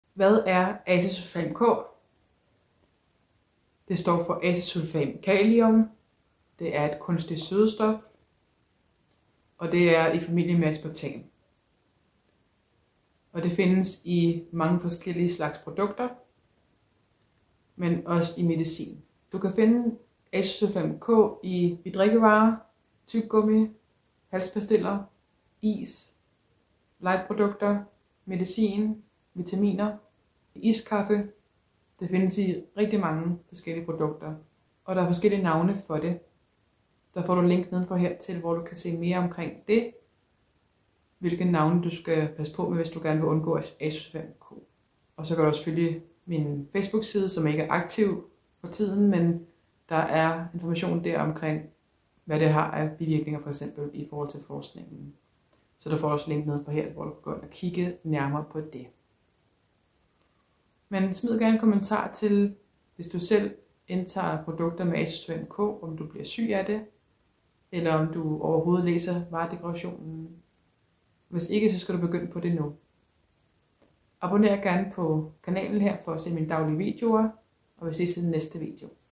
SØDESTOFFET DU SKAL UNDGÅ researcher fortæller - The sweetener you should avoid Researcher explain
Hvad er Acesulfam-k og hvor findes det?

Læs mere på min hjemmeside om andre kunstige sødestoffer og meget mere